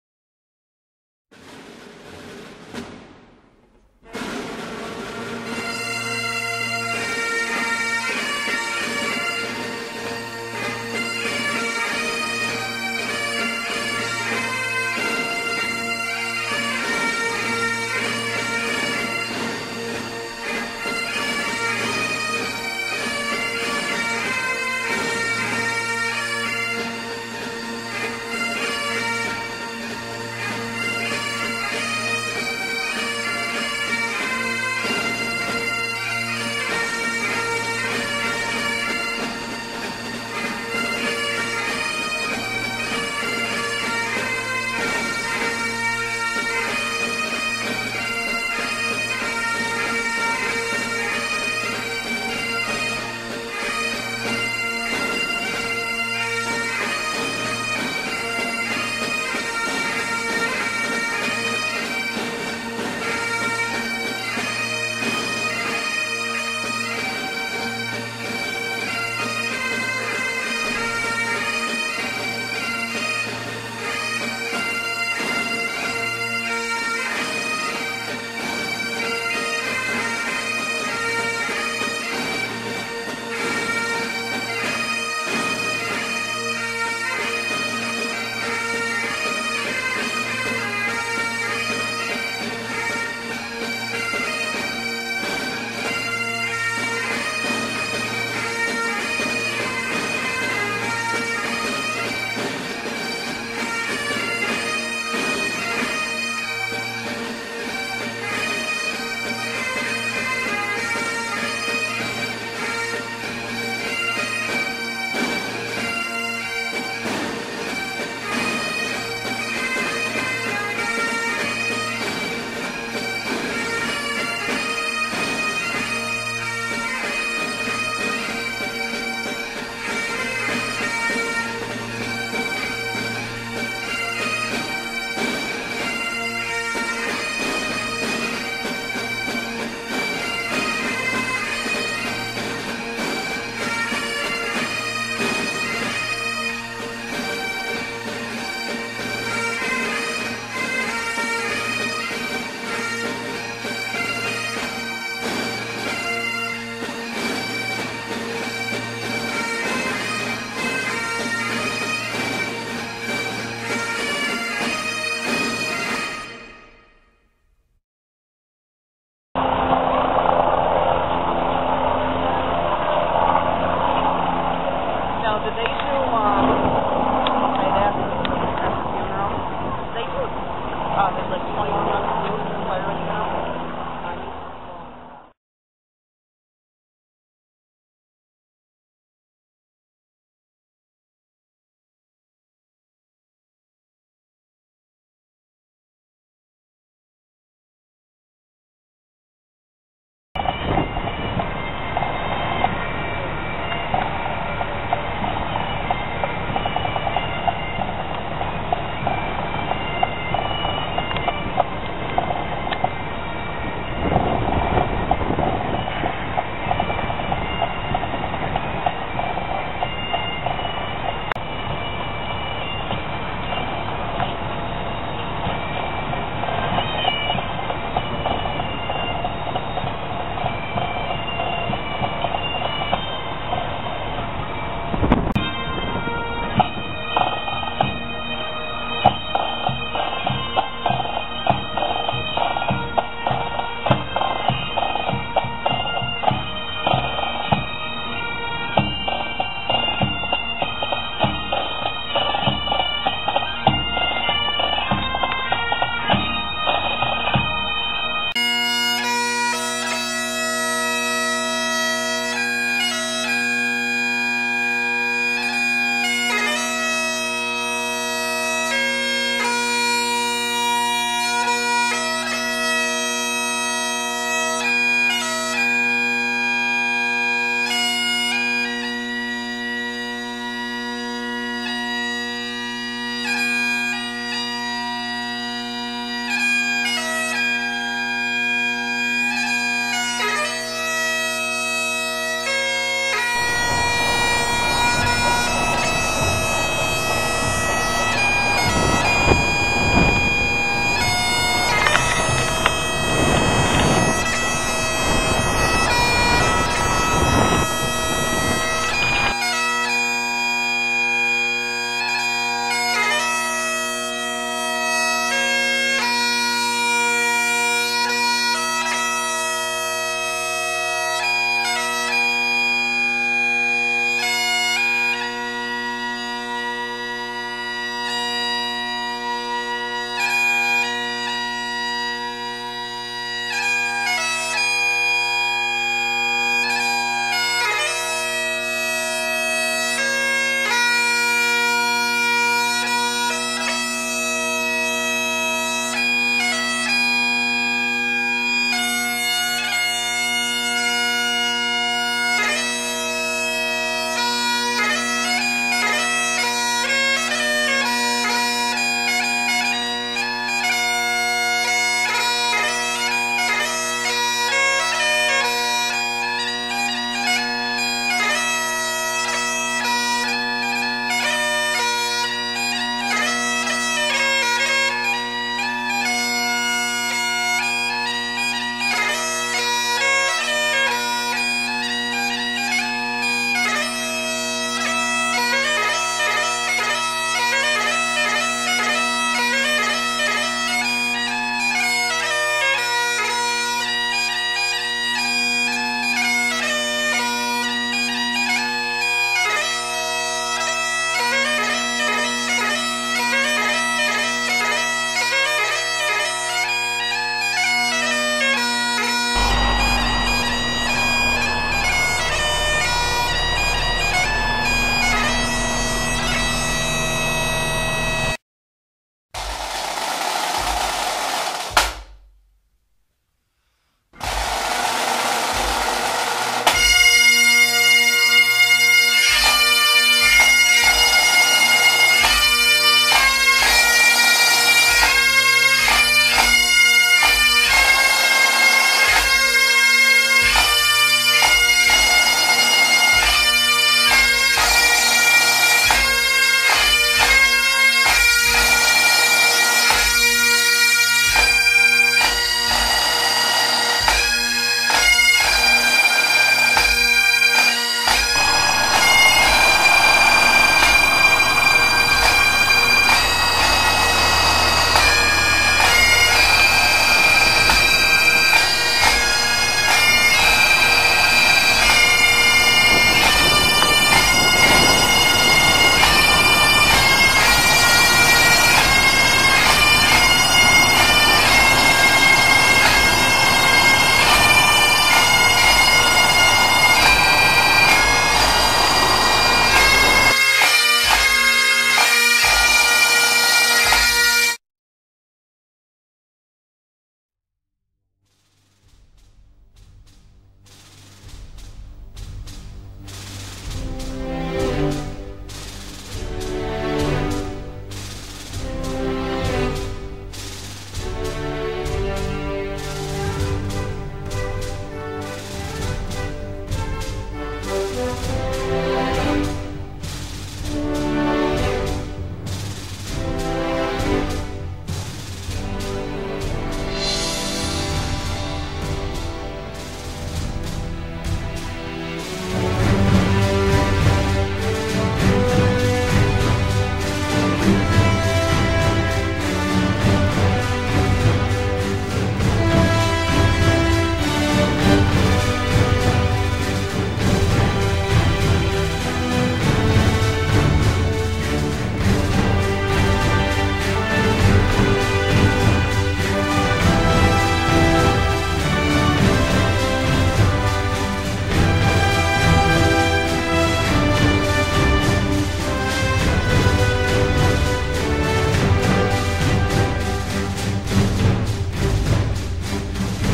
A tribute to Mark Davis
A tribute to Mark Davis who was killed in the line of duty doing what he loved to do on Jan 31st 2009. Rest in peace brother.